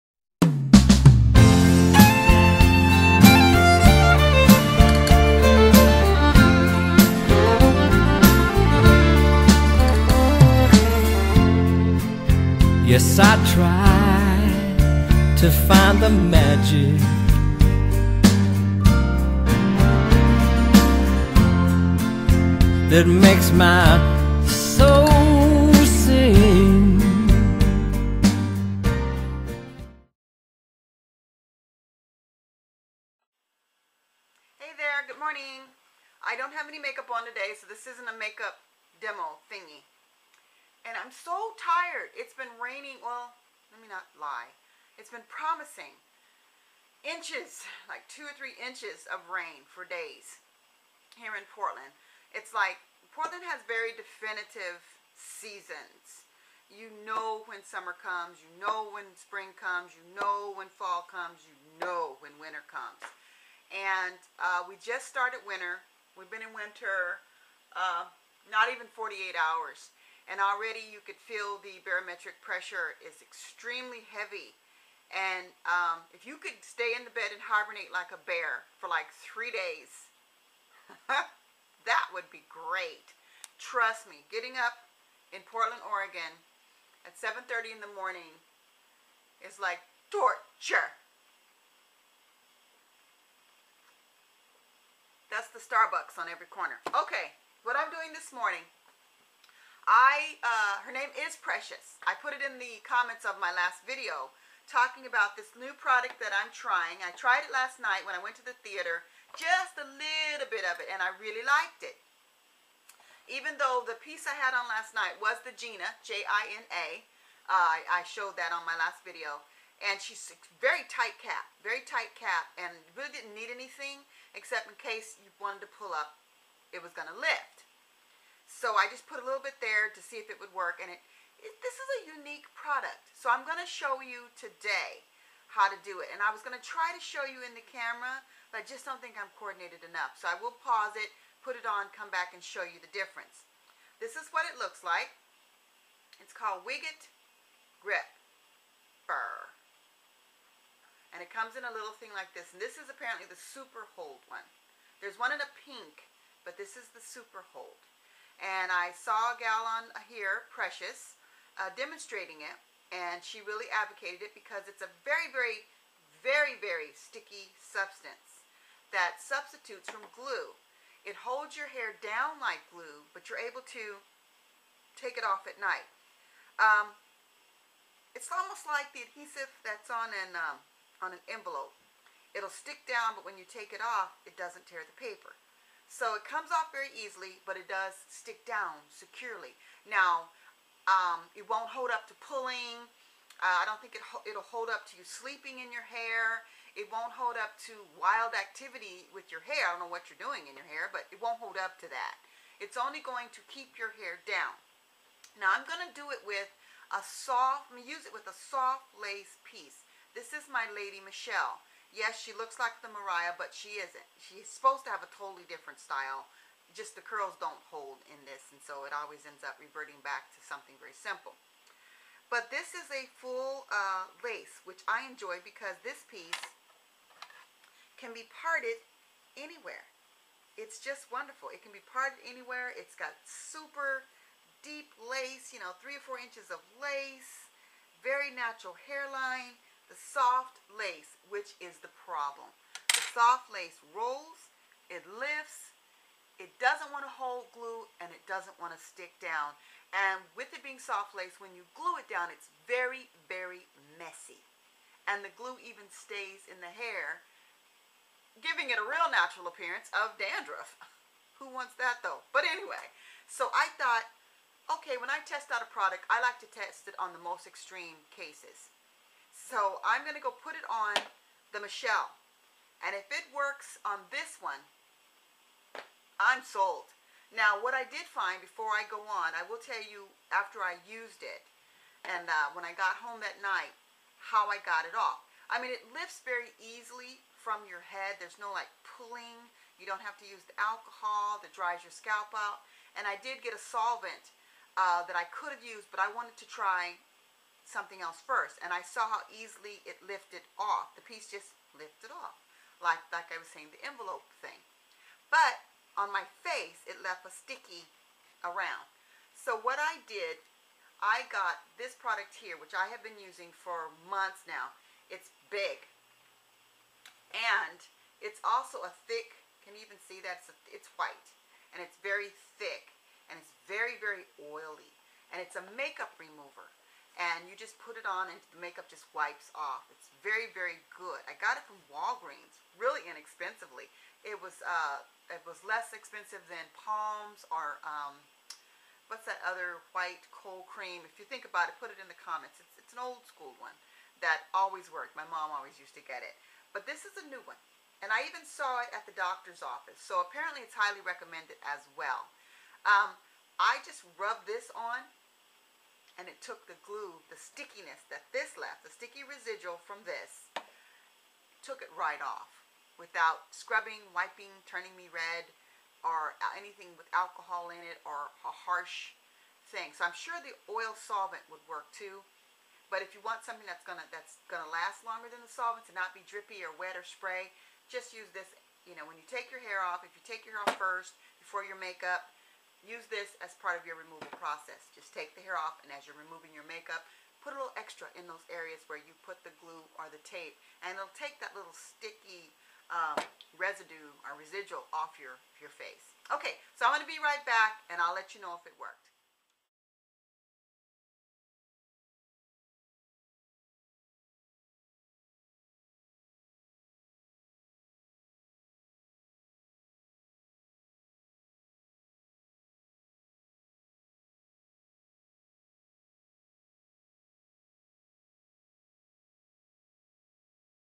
Product Review: Wig It Gripper pt 1
Testing the product on my most challenging piece I'd have to give it a B- for hold. The Gripper does not dry or permanently hold hair down. It is a temporary quick fix for lace front. It will hold your hair in place such as baby hair and the like.
You can get @ Elevatedsyle.com and on Amazon. Shipping was only 2 days on Amazon.